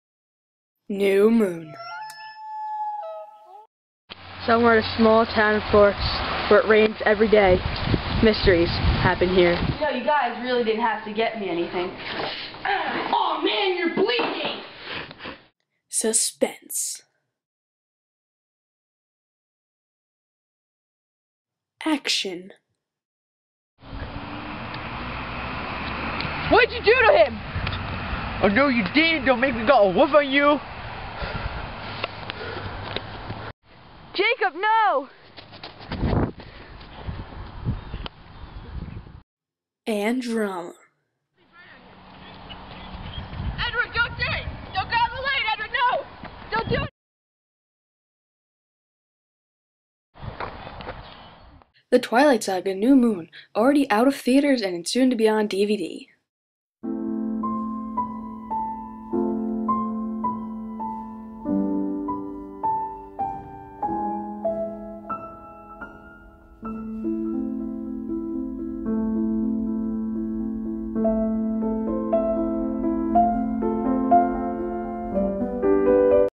new moon parody
a movie i had to do for a project for english class. this could have been better if i was aloud to curse in it but it was for school so i couldnt. :(